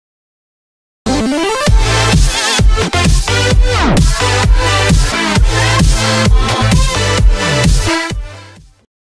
Abstract Logo Opener (Basic Package)
Abstract Logo Opener for your brand and YouTube.
Available in Every Resolution. (720p, 1080p, 4k)
Changeable Colors.